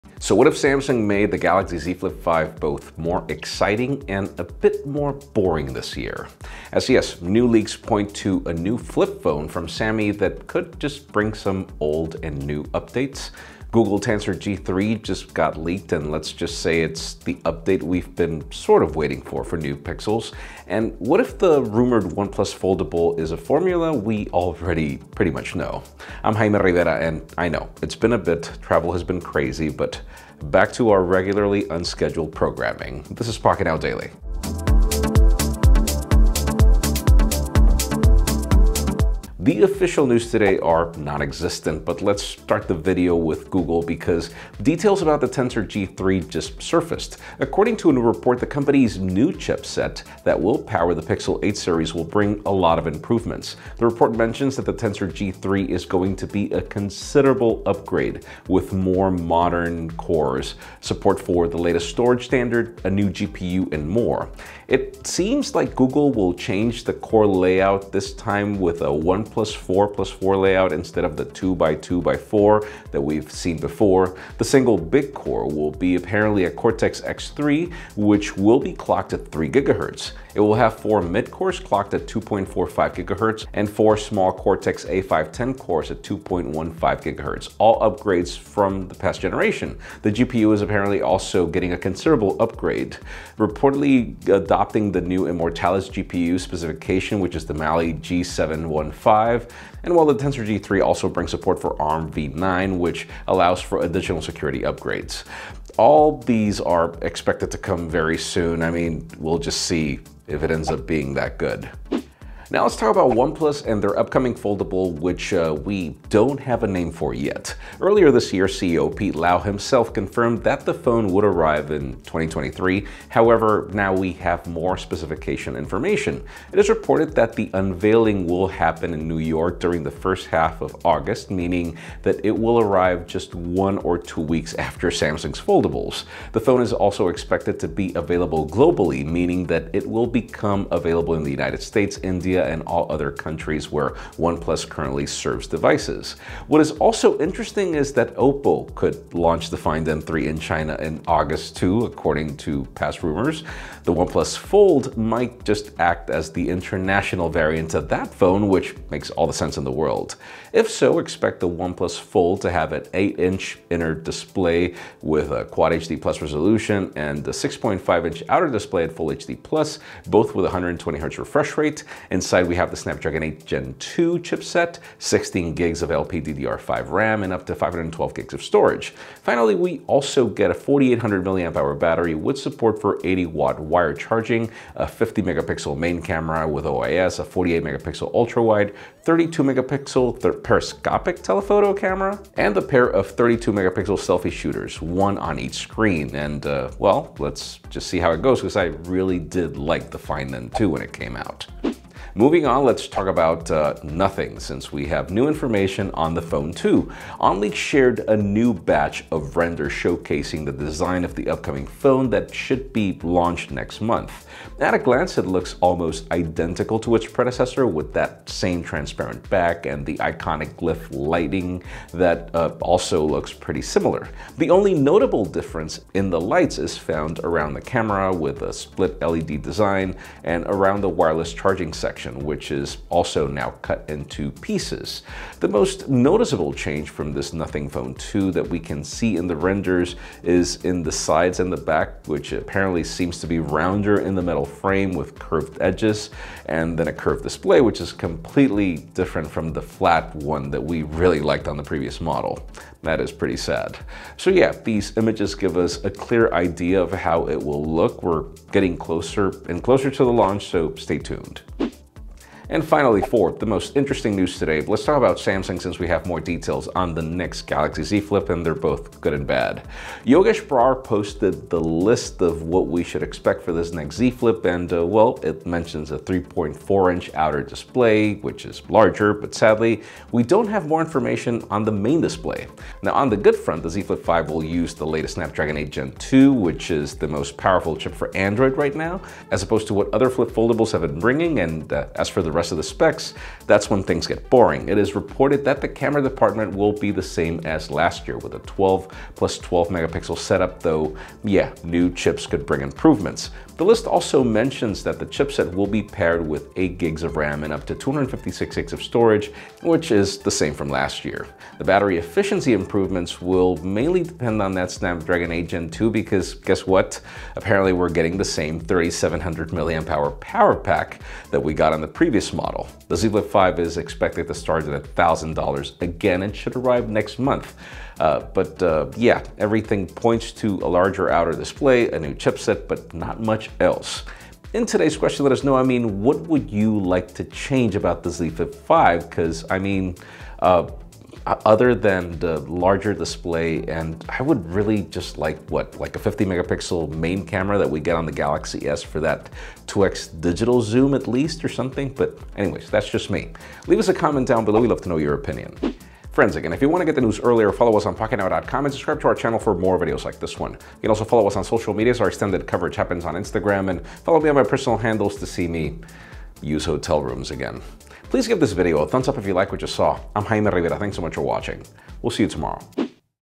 Galaxy Z Flip 5: Most BORING Update? Nothing Phone 2 Renders & more!
On Pocketnow Daily, Google’s Tensor G3 just got leaked and let’s just say it’s the update we’ve been waiting for on the next Pixels. And what if that rumored OnePlus Foldable is a formula we pretty much already know? We have new renders showcasing the Nothing Phone 2. Finally,  what if Samsung made the Galaxy Z Flip 5 more exciting and boring this year?

Stories: 0:00 - Intro
0:39 - Google Tensor G3
1:52 - OnePlus Fold
3:21 - Nothing Phone 2 Renders
4:21 - Galaxy Z Flip 5 Specs

Today’s Deals:
 - 2021 16-inch MacBook Pro
- Samsung 8K Smart TV
- Pixel 7 Pro
 - AirPods Max